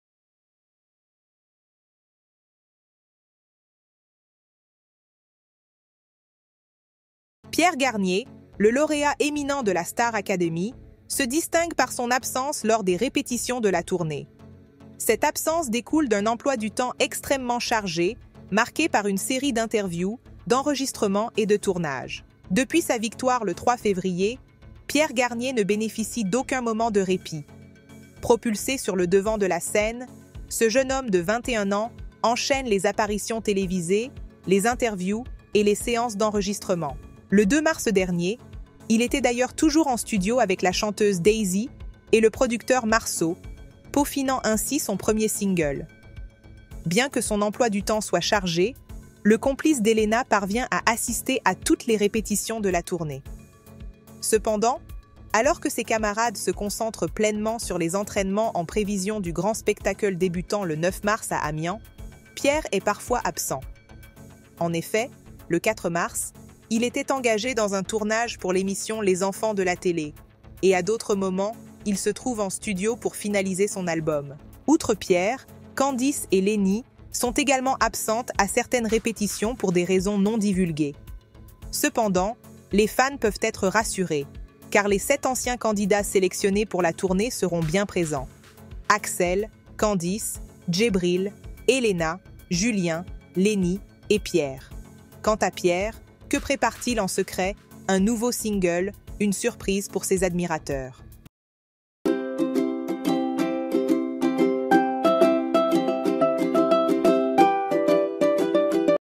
Star Academy : Pierre Garnier, absent des répétitions ? Les raisons de son absence dévoilées
Pierre Garnier, le lauréat éminent de la Star Academy, se distingue par son absence lors des répétitions de la tournée. Cette absence découle d'un emploi du temps extrêmement chargé, marqué par une série d'interviews, d'enregistrements et de tournages. Depuis sa victoire le 3 février, Pierre Garnier ne bénéficie d'aucun moment de répit. Propulsé sur le devant de la scène, ce jeune homme de 21 ans enchaîne les apparitions télévisées, les interviews et les séances d'enregistrement.

Le 2 mars dernier, il était d'ailleurs toujours en studio avec la chanteuse Daysy et le producteur Marso, peaufinant ainsi son premier single. Bien que son emploi du temps soit chargé, le complice d'Helena parvient à assister à toutes les répétitions de la tournée. Cependant, alors que ses camarades se concentrent pleinement sur les entraînements en prévision du grand spectacle débutant le 9 mars à Amiens, Pierre est parfois absent. En effet, le 4 mars, il était engagé dans un tournage pour l'émission "Les enfants de la télé", et à d'autres moments, il se trouve en studio pour finaliser son album.